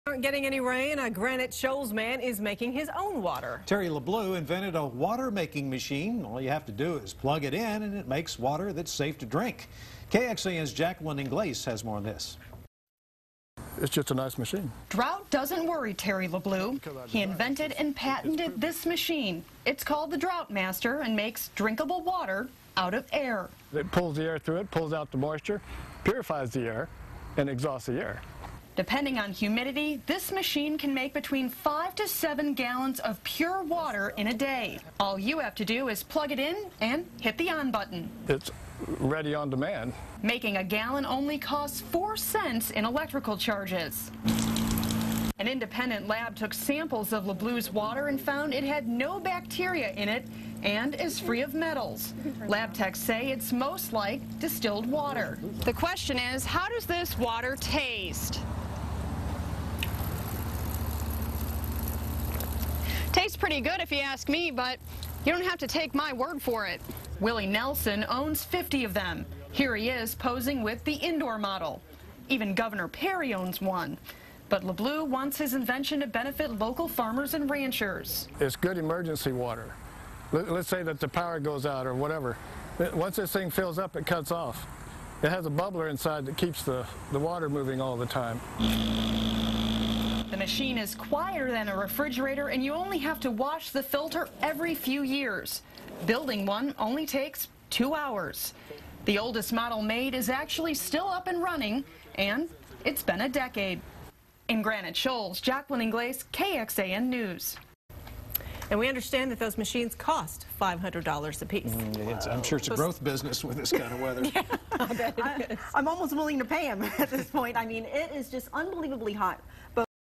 KXAN-Granite Shoals man makes water
Inventor Terry LeBleu has made a machine that can make water from air.